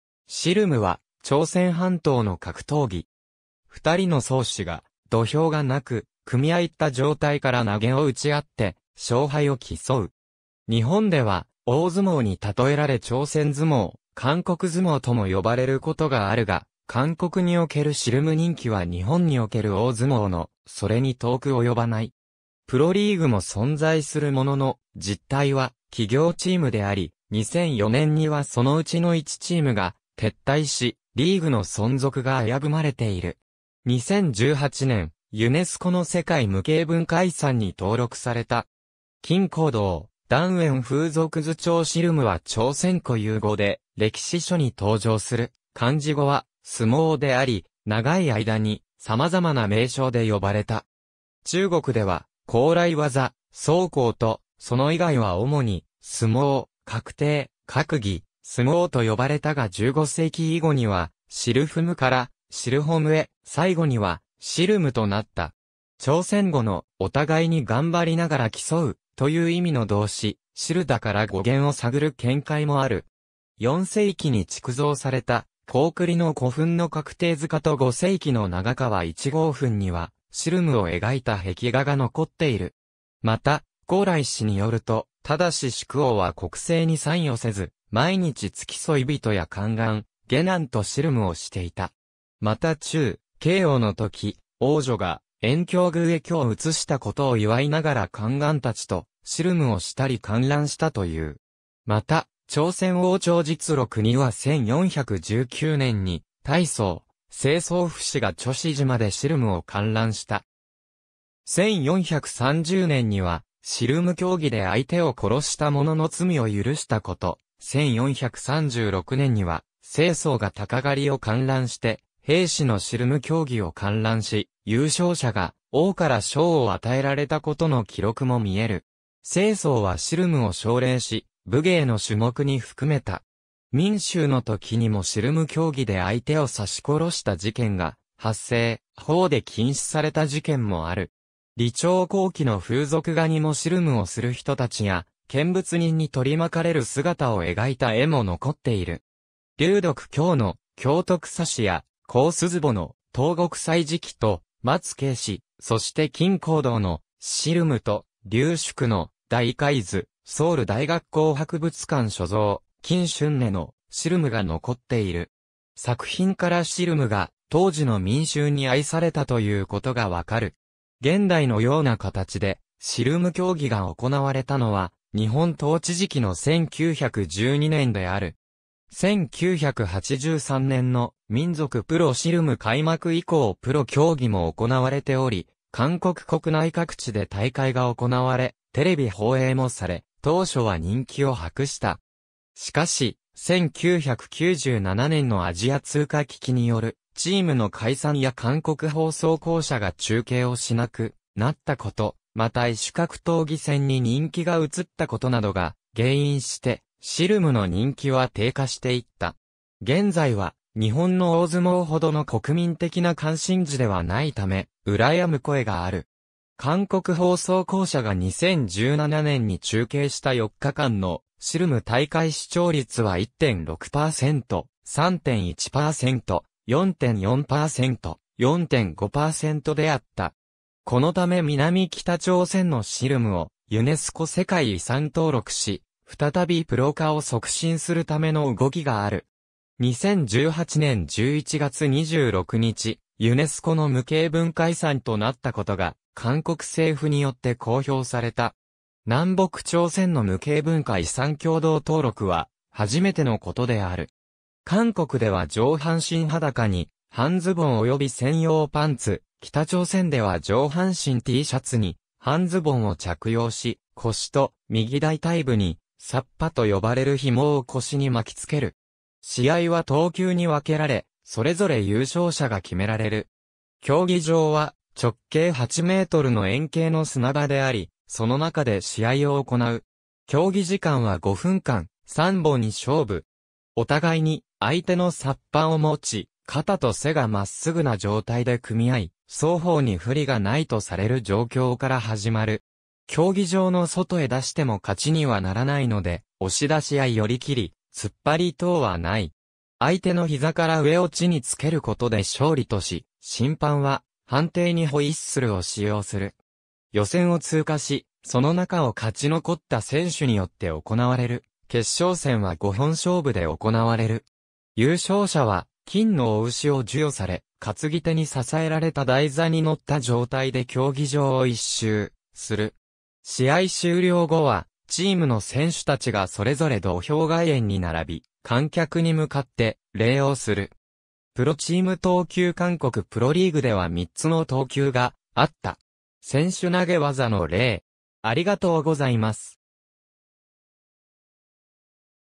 シルム
シルム（씨름）は朝鮮半島の格闘技。
二人の壮士（チャンサ:シルムにおける競技者の呼び名）が、土俵がなく、組合った状態から投げを打ち合って勝敗を競う。
日本では大相撲にたとえられ朝鮮相撲、韓国相撲とも呼ばれることがあるが、韓国におけるシルム人気は日本における大相撲のそれに遠く及ばない。
プロリーグも存在するものの、実態は企業チームであり、2004年にはそのうちの1チームが撤退し、リーグの存続が危ぶまれている。
2018年ユネスコの世界無形文化遺産に登録された。
金弘道『檀園風俗図帖』（1780年頃） シルムは朝鮮固有語で歴史書に登場する漢字語は「角力」であり、長い間に様々な名称で呼ばれた。
中国では「高麗技」「争交」と、その以外は主に「角力」「角抵」「角戯」「相撲」と呼ばれたが15世紀以後には「シルフム」から「シルホム」へ、最後には「シルム」となった。
朝鮮語の「お互いに頑張りながら競う」という意味の動詞「シルダ」から語源を探る見解もある。
4世紀に築造された高句麗の古墳の角抵塚と5世紀の長川1号墳にはシルムを描いた壁画が残っている。
また『高麗史』によると、忠粛王は国政に参与せず、毎日付き添い人や宦官・下男とシルムをしていた。
また忠恵王の時、王女が延慶宮へ居を移したことを祝いながら宦官たちとシルムをしたり観覧したという。
また 『朝鮮王朝実録』には1419年に太宗・世宗 父子が楮子島でシルムを観覧した。
1430年にはシルム競技で相手を殺した者の罪を許したこと、 1436年には世宗が鷹狩りを観覧して、兵士のシルム競技を観覧し、優勝者が王から賞を与えられたことの記録も見える。
世宗はシルムを奨励し、武芸の種目に含めた。
明宗の時にもシルム競技で相手を刺し殺した事件が発生、法で禁止された事件もある。
李朝後期の風俗画にもシルムをする人たちや見物人に取り巻かれる姿を描いた絵も残っている。
柳得恭の『京都雜志』や洪錫謨の『東国歳時記』と『松京誌』、そして金弘道の『シルム』と劉淑の『大快図』(ソウル大学校博物館所蔵）金俊根の『シルム』が残っている。
作品からシルムが当時の民衆に愛されたということがわかる。
現代のような形でシルム競技が行われたのは日本統治時期の1912年である。
1983年の「民俗プロ・シルム」開幕以降プロ競技も行われており、韓国国内各地で大会が行われ、テレビ放映もされ、当初は人気を博した。
しかし、1997年のアジア通貨危機によるチームの解散や韓国放送公社が中継をしなくなったこと、また異種格闘技戦に人気が移ったことなどが原因して、シルムの人気は低下していった。
現在は日本の大相撲ほどの国民的な関心事ではないため、羨む声がある。
韓国放送公社が2017年に中継した4日間のシルム大会視聴率は1.6％、3.1％、4.4％、4.5％であった。
このため南北朝鮮のシルムをユネスコ世界遺産登録し、再びプロ化を促進するための動きがある。
2018年11月26日、ユネスコの無形文化遺産となったことが韓国政府によって公表された。
南北朝鮮の無形文化遺産共同登録は初めてのことである。
韓国では上半身裸に半ズボン及び専用パンツ、北朝鮮では上半身Tシャツに半ズボンを着用し、腰と右大腿部に「サッパ」 と呼ばれる紐を腰に巻きつける。
試合は等級（重量別）に別けられ、それぞれ優勝者が決められる。
競技場は、直径8mの円形の砂場 であり、その中で試合を行う。
競技時間は5分間、3本勝負。
お互いに相手のサッパを持ち、肩と背が真っ直ぐな状態で組み合い、双方に不利が無いとされる状況から始まる。
競技場の外へ出しても勝ちにはならないので、押し出しや寄り切り、突っ張り等は無い。
相手の膝から上を地につけることで勝利とし、審判は、判定にホイッスルを使用する。
予選を通過し、その中を勝ち残った選手によって行われる決勝戦は5本勝負で行われる。
優勝者は金の雄牛を授与され、担ぎ手に支えられた台座に乗った状態で競技場を一周する。
試合終了後はチームの選手達がそれぞれ土俵外縁に並び、観客に向かって礼をする。
プロチーム（韓国） 等級（韓国プロリーグ） 韓国プロリーグでは3つの等級があった。
選手 投げ技の例